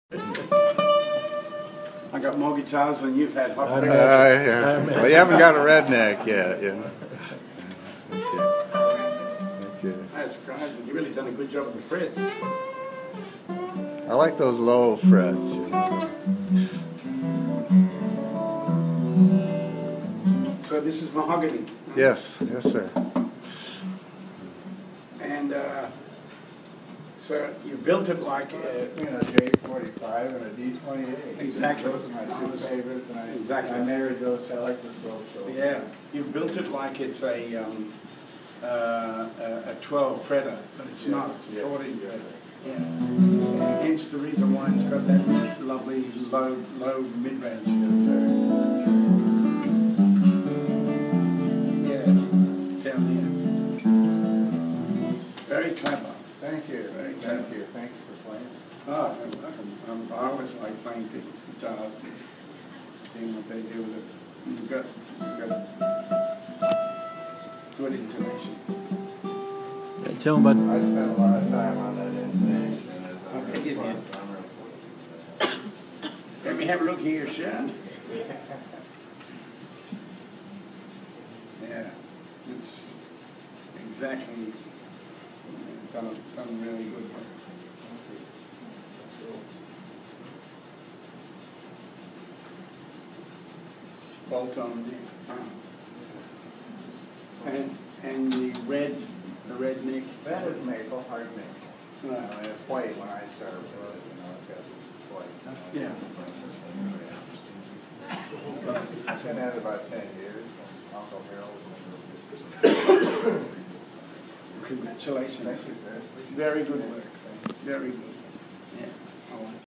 Tommy Emmanuel - Critiques Redneck Guitar
During the Meet & Greet session at The Clayton Center, Clayton, NC.  9/22/12. Tommy played the Redneck later in the session. ..also on You Tube.